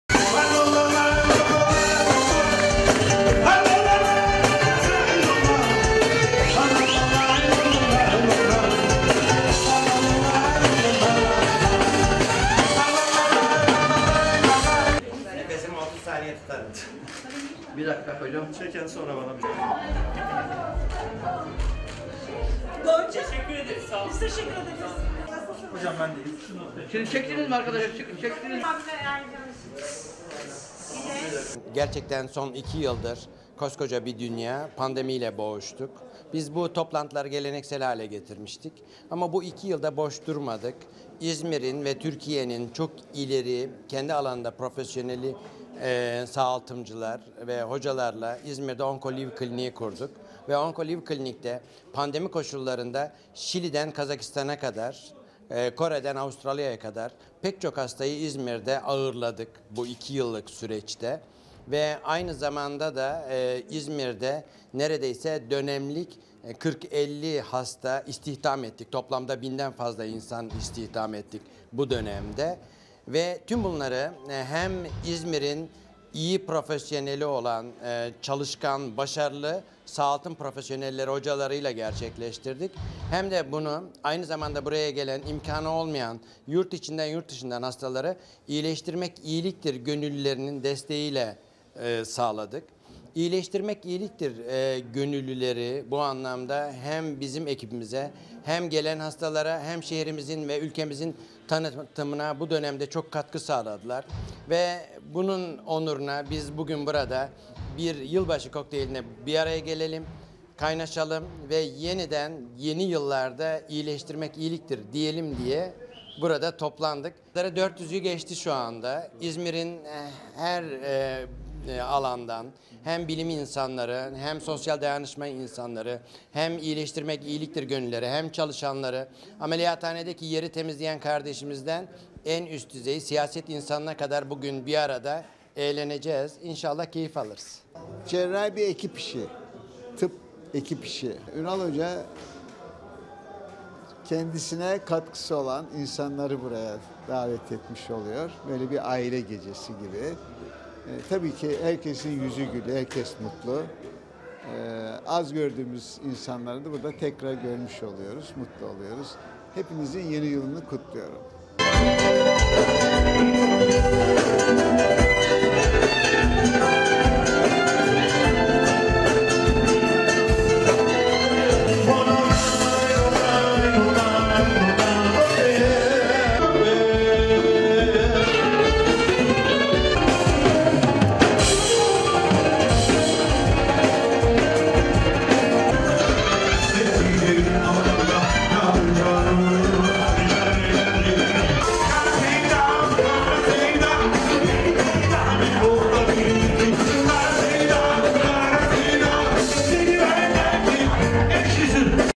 İzmir’de sağlık çalışanları yılbaşı yemeğinde buluştu
İzmir’de yaklaşık 400 sağlık çalışanı ve bilim insanı, yılbaşı yemeğinde bir araya geldi. Elde edilen gelirin kanser hastalarına bağışlanacağı etkinliği organize eden İyileştirmek İyiliktir Derneği Kurucu Başkanı Genel Cerrah Prof. Dr. Ünal Aydın, pandemiye rağmen İzmir’de yurt içi ve yurt dışından birçok hastayı ağırladıklarını belirterek, İzmir’in ve Türkiye’nin tanıtımına katkı sağladıklarını söyledi. Ünal Aydın, dernek çatısı altında 5 yıldır kanser hastalarına psikolojik, ekonomik ve sosyolojik anlamda destek verdiklerini ifade etti.
İzmir’de 2016 yılında kurulan ‘İyileştirmek İyiliktir Derneği’, kanser hastalıklarının bütüncül tedavisi için çalışıyor. Dernek tarafından, geliri kanserli hastaların yararına kullanılmak üzere bir yeni yıl yemeği düzenlendi. Oncolive Klinik’in katkılarıyla düzenlenen gecede Tepecik Senfoni Orkestrası sahne alırken, İzmir’in akademik, sosyal ve sanat camiasından birçok isim yer aldı.